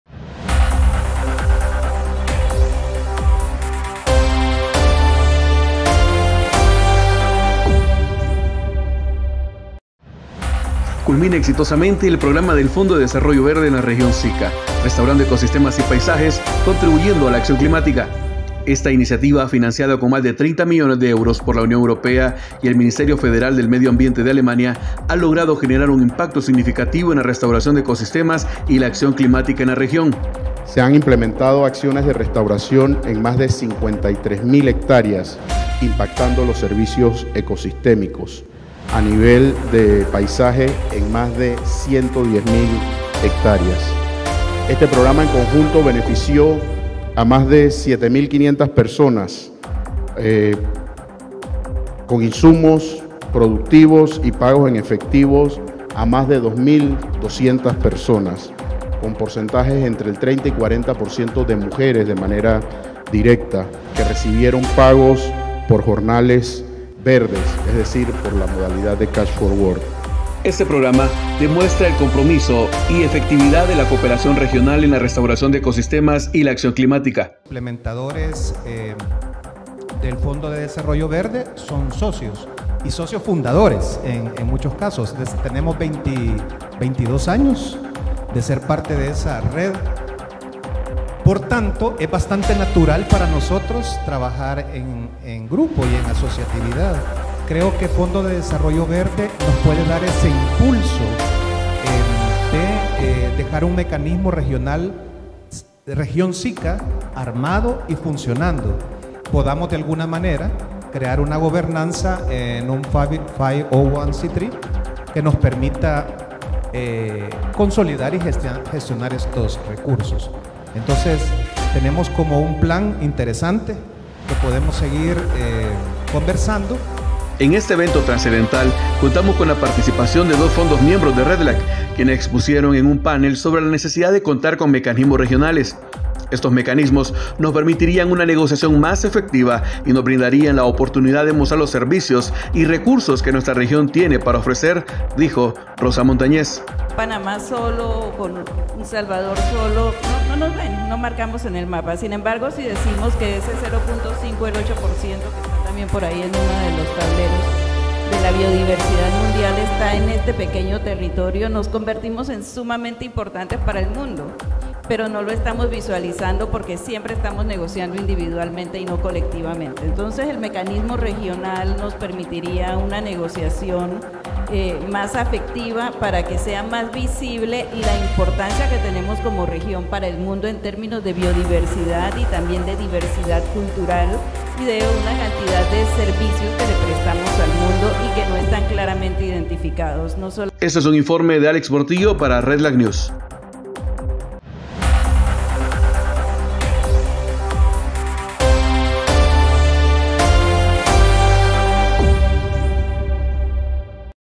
RedLAC News, primera entrega. "Cierre Regional del @restaurandoecosistemas ".
RedLAC|| 📢 ¡Bienvenidos a RedLAC News! 🌍

Estamos emocionados de presentarles nuestra primera entrega. En esta ocasión, les traemos el emocionante Cierre Regional del Fondo de Desarrollo Verde para la región SICA / REDD+ Landscape 🌿🌱
Los miembros de RedLAC se unieron para promover la conservación y el desarrollo sostenible en la región. Este hito trascendental demuestra el compromiso de la alianza en la implementación de proyectos ambientales innovadores 💪💚

No pueden perderse nuestras próximas entregas, donde seguiremos informando sobre los avances y proyectos más destacados en la región. ¡Únanse a nosotros en la construcción de un futuro más verde y sostenible! 🌿🌎